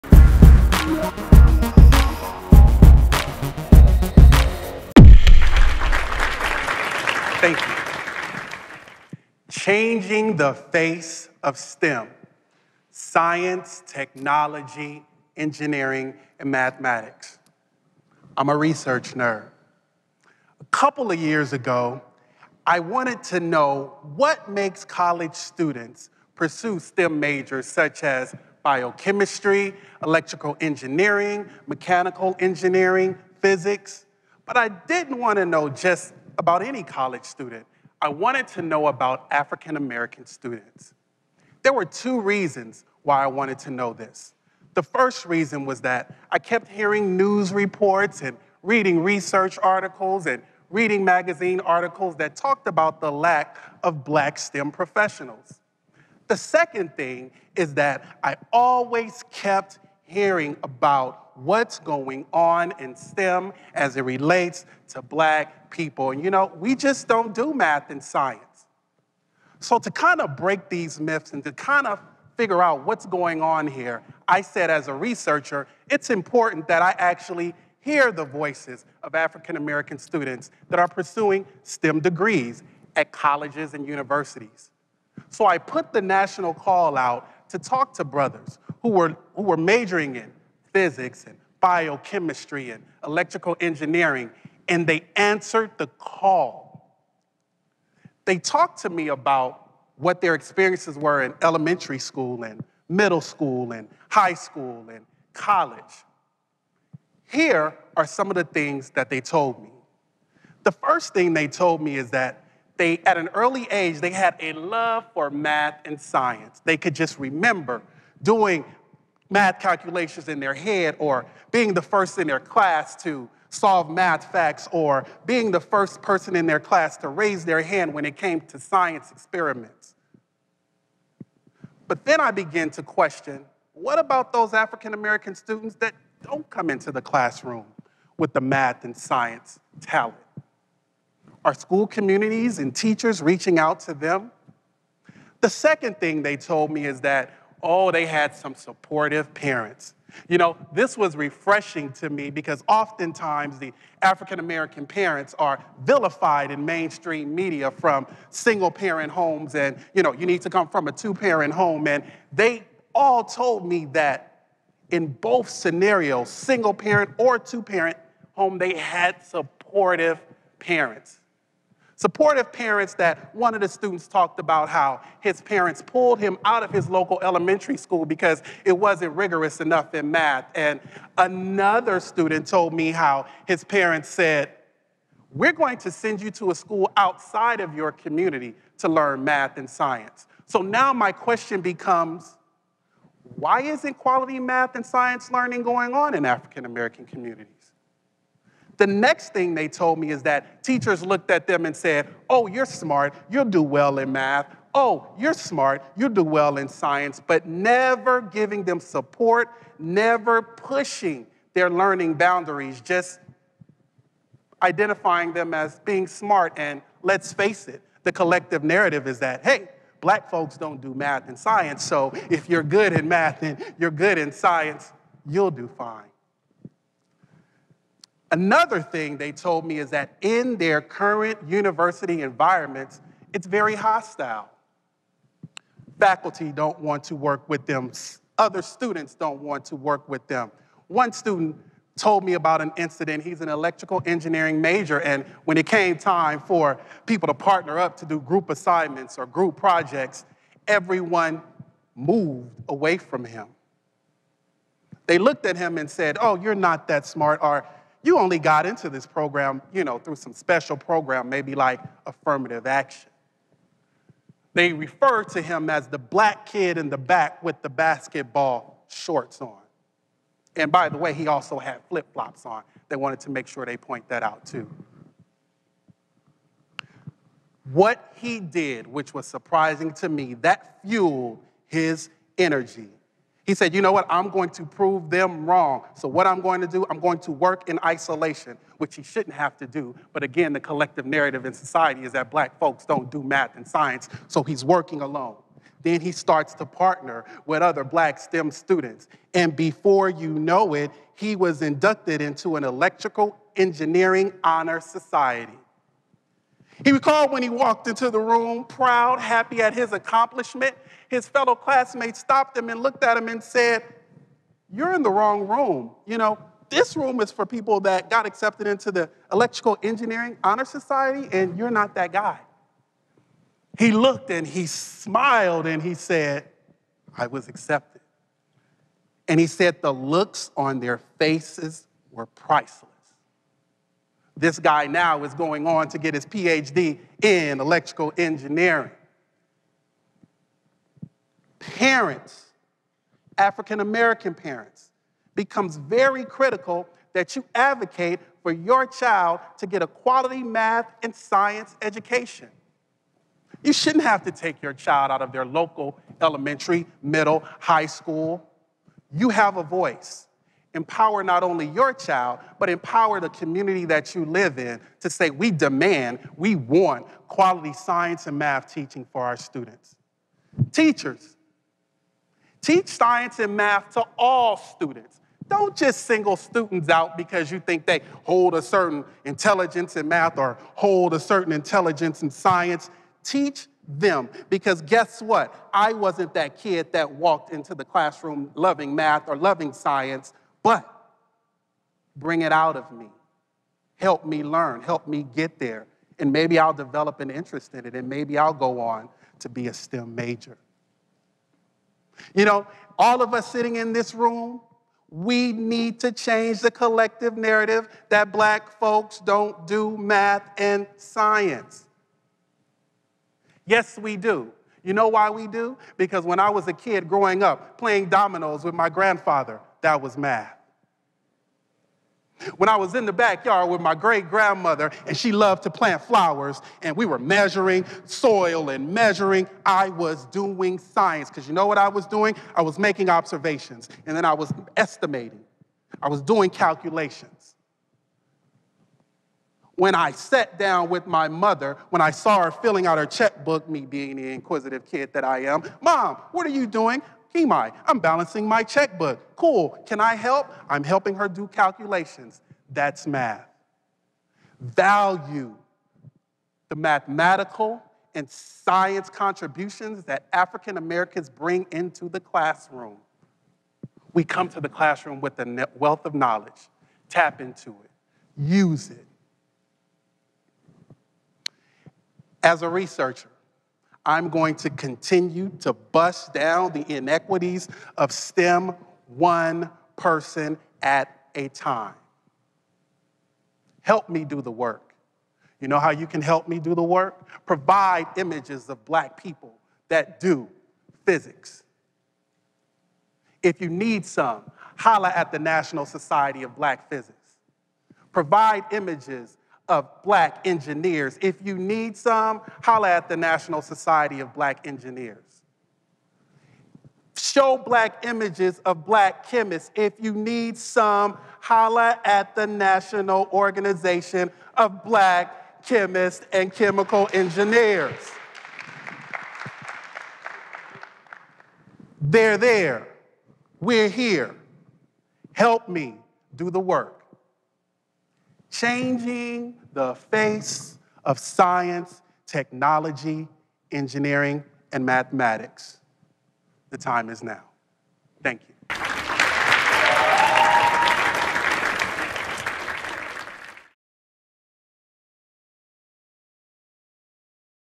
Changing the Face of STEM | KiMi Wilson | TEDxCrenshaw
KiMi Wilson speaks at a 2015 TEDx event in Los Angeles, California.

Wilson is a former K-12 teacher, and former Education and Outreach co- ordinator for the Center for Scalable and Integrated Nanomanufacturing research center at UCLA.